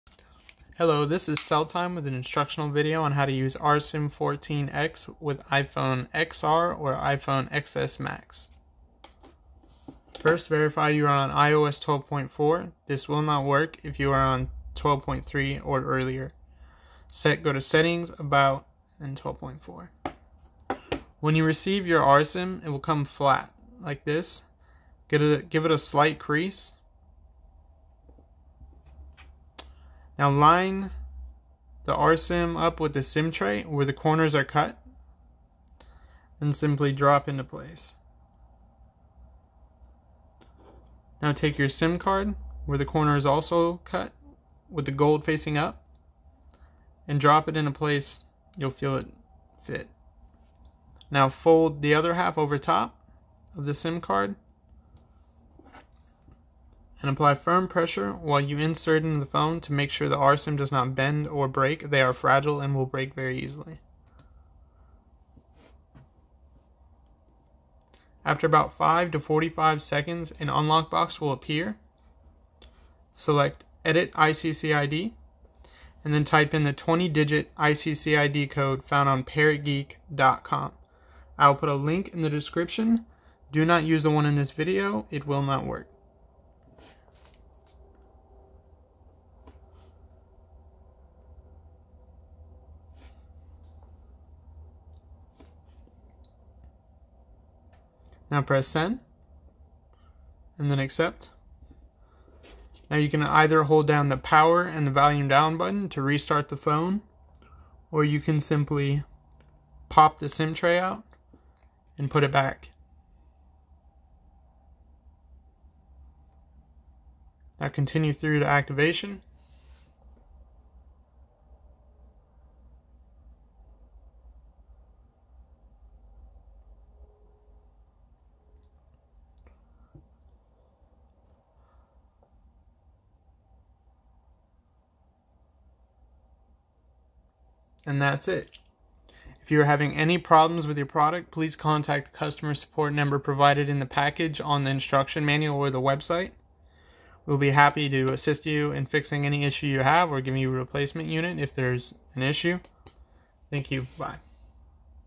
How to use RSIM 14 with Iphone Xr & Xs Max IOS 12.4
First verify you are running IOS 12.4 or Newer this will not work on IOS 12.3 or Earlier.

​1. Insert RSIM with the sim card you want to use into the phone (be careful Chip is Fragile)

2. After about 5- 45 Seconds an Auto Unlock dialog box will appear

4 select Edit ICCID

5. Find the new current 20 digit iccid Code from   parrotgeek.com

Enter this and then press Send

 ​6. Now press okay/Accept and RESTART the phone then continue through to the home screen and you should have service :)

Chip must remain in the phone at all times! RSIM only works with GSM Sim Cards

To purchase goto Getrsim.com